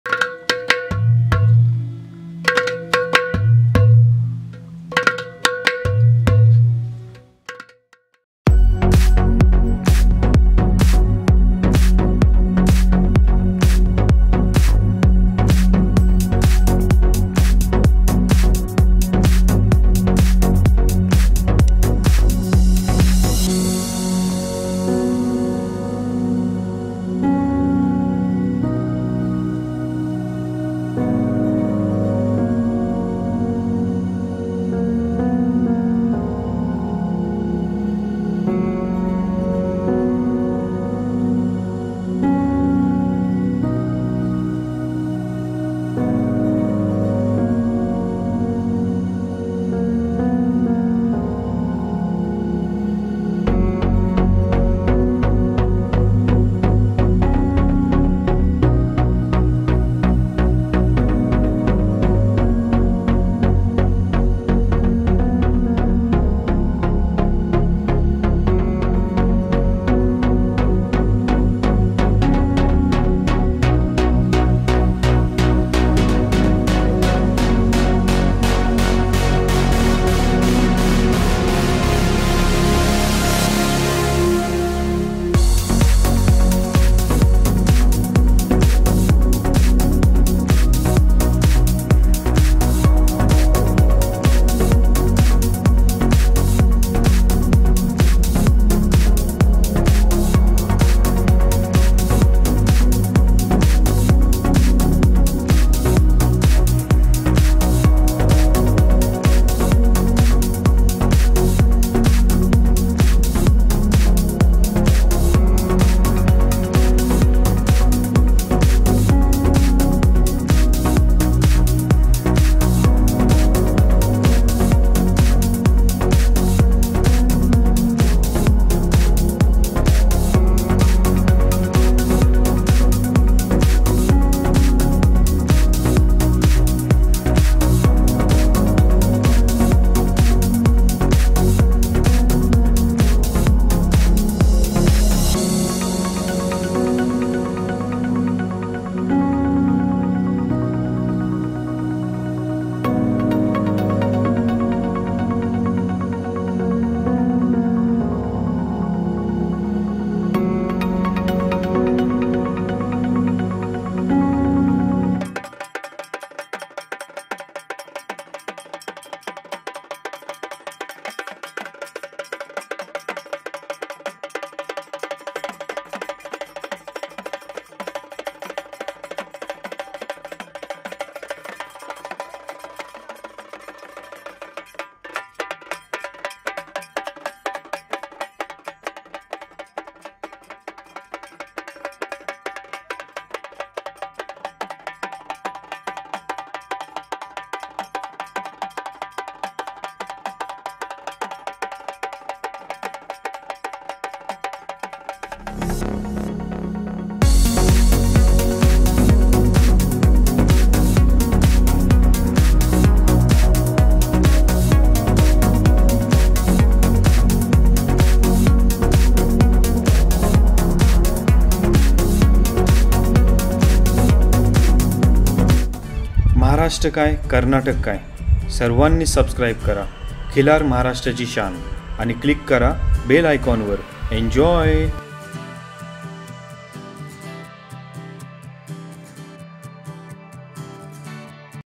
Khillar Maharashtrachi Shaan | घटप्रभा २०१९ मधील सर्वात मोठी खरेदी
*खिल्लार महाराष्ट्राची शान* चॅनलवर आज आपण पाहणार आहोत, घटप्रभा २०१९ मधील सर्वात मोठी खरेदी
*अक्षय राजुशेठ गिरी, सातारा* यांच्या सहकार्याने *सागर टिळेकर, पुणे* यांनी केलेली खरेदी ! आजच्या भागात या बैलांबद्दल माहिती आपण पाहणार आहोत.